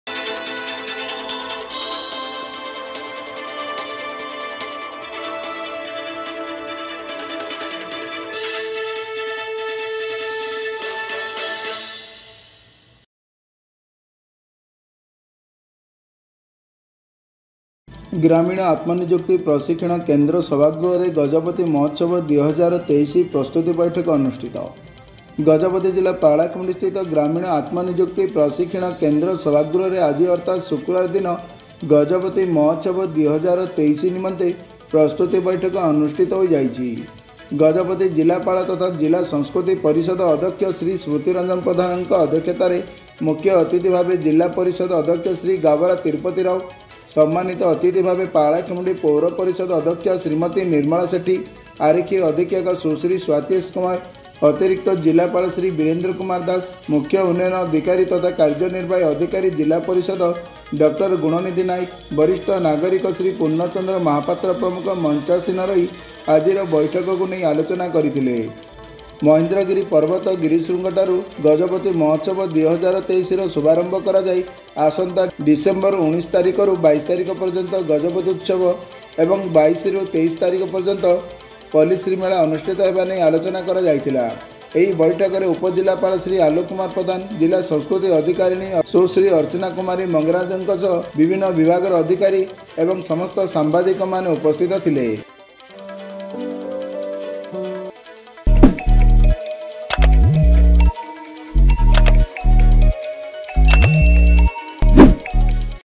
ଆସନ୍ତା ଡ଼ିସେମ୍ବର ୧୯ ତାରିଖ ଠାରୁ ୨୨ ଯାଏ ହେବ ଗଜପତି ଉତ୍ସବ ୨୦୨୩
ak14tv 20/10/2023